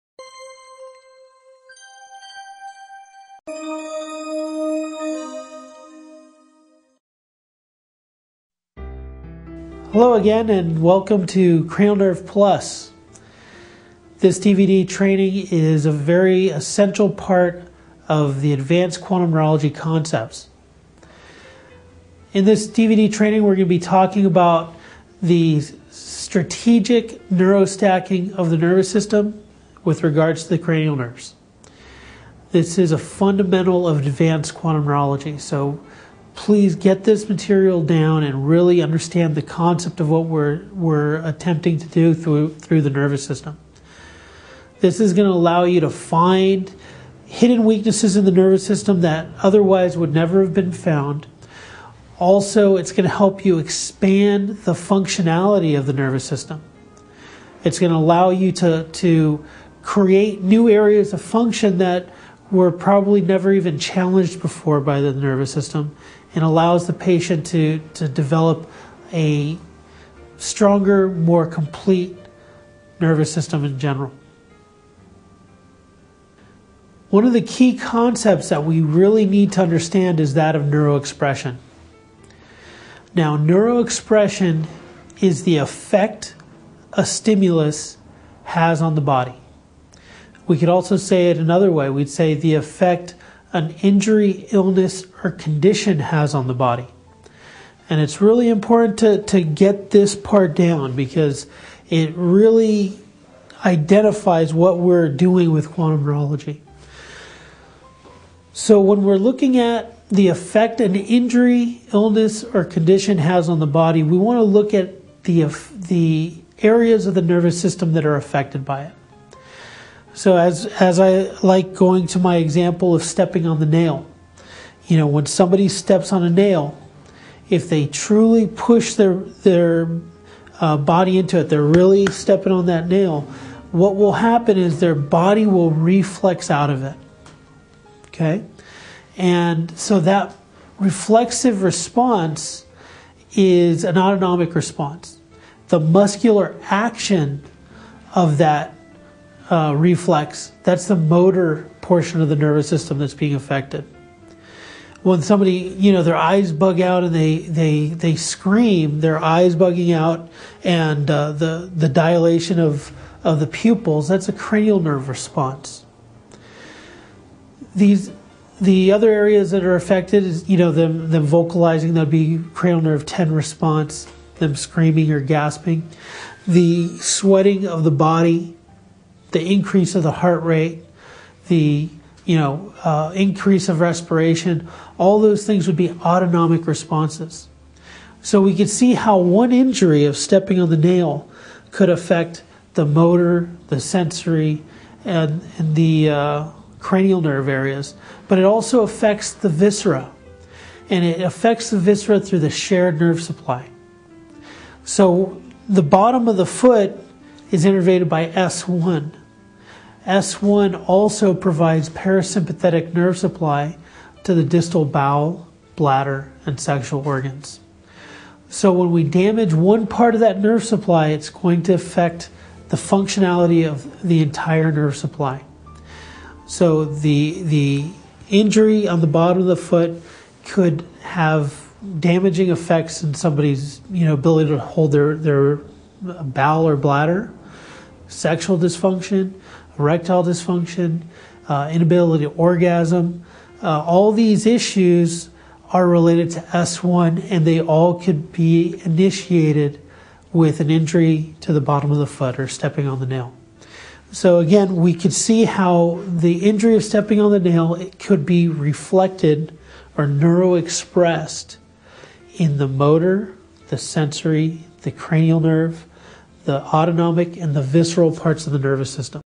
Cranial Nerve Plus DVD - Intro
Take your Cranial Nerve work to the next level. In this DVD, Dr. Gonzalez presents a unique concept that is beyond the correction of a weakened nerve. This training provides a method to evaluate, identify an expand the capabilities of the Nervous System. This DVD contains the fundamental skills that are needed in order to achieve advanced corrections in Quantum Neurology®.